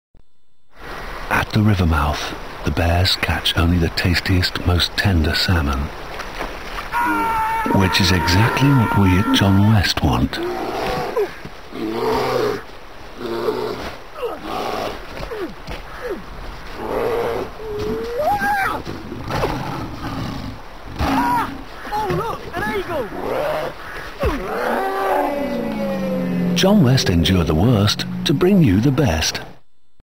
fisher attacks a bear for fish
very funny he attacks near a lake